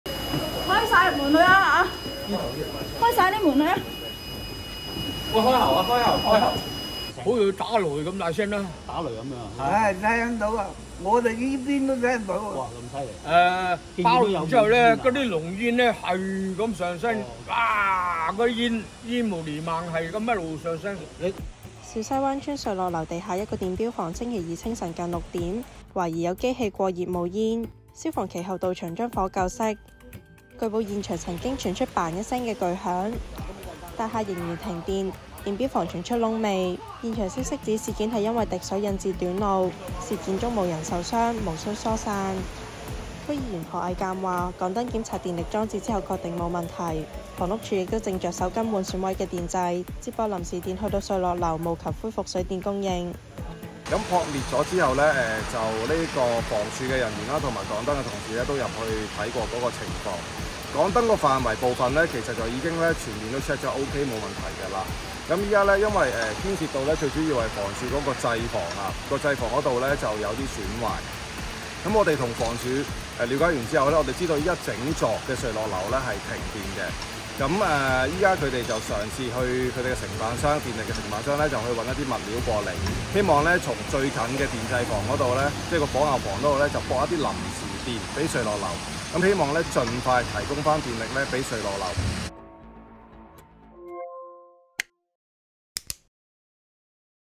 小西灣邨瑞樂樓電錶房傳巨響 大廈停電 濃煙席捲半空｜突發｜小西灣｜火警｜爆炸｜電錶房
小西灣邨瑞樂樓電錶房傳巨響　大廈停電　濃煙席捲半空

小西灣邨發生火警。今日（4日）清晨5時51分，小西灣邨瑞樂樓地下一個電錶房內，懷疑有機器過熱冒煙，大量濃煙席捲半空，消防其後到場救熄。據報現場曾傳出「砰」一聲巨響，大廈仍然停電，電錶房傳出燶味，現場消息指事件因滴水引致短路。事件中無人受傷，毋須疏散。